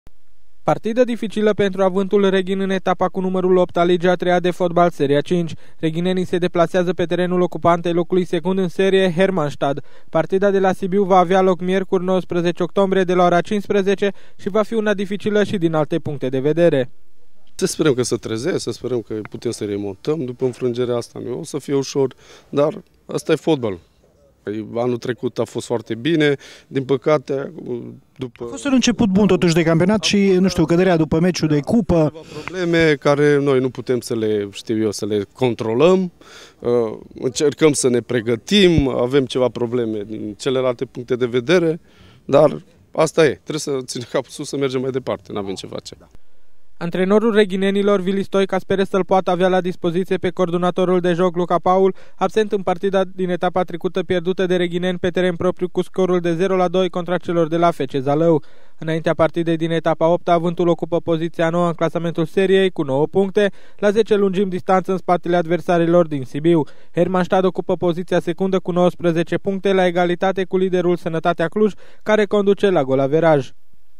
DEPLASARE DIFICILĂ PENTRU REGHINENI (2016 10 18)
Deplasare dificilă pentru Avântul Reghin în etapa a 8-a a ligii a treia de fotbal seria 5. Elevii antrenaţi de Vili Stoica întâlneşte din postura de echipă oaspete, ocupanta locului secund în serie, Hermannstadt.  Înaintea meciului de la Sibiu, Avântul ocupă locul nouă cu 9 puncte dar şi cu un meci mai puţin disputat. Meciul de la Sibiu va avea loc miercuri, 19 Octombrie de la ora 15.